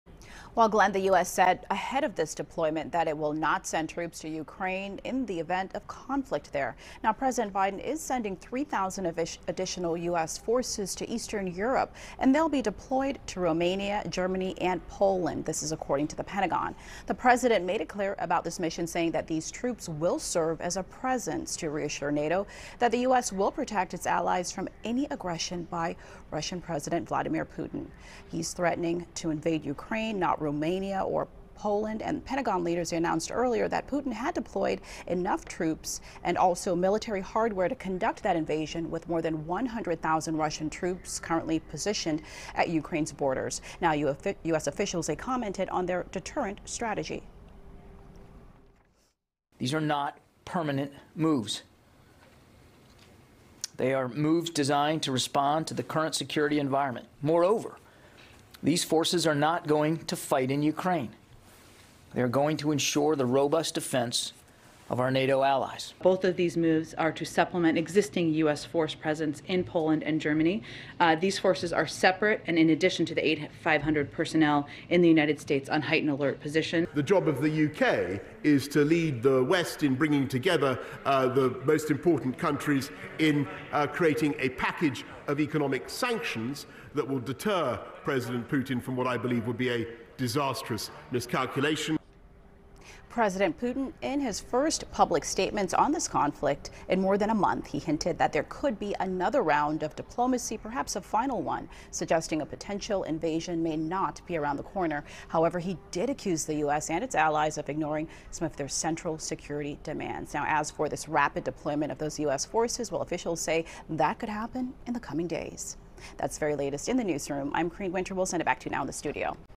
More U.S. troops deployed to Europe amid Ukraine tension
Kareen Wynter reports for the KTLA 5 News at Noon on Feb. 2, 2022.